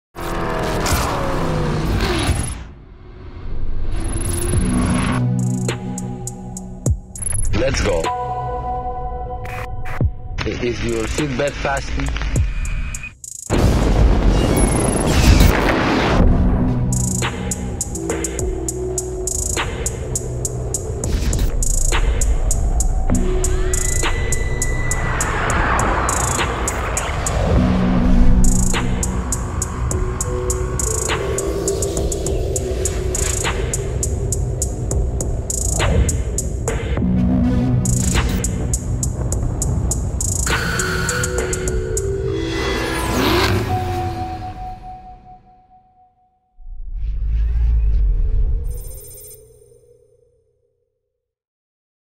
Breaking Down The Next Generation Corvette Teaser - Sanger Chevrolet
The Next Generation Corvette teaser video has more than 60 years of mid-engine history and heritage hidden within its 07.18.19 length.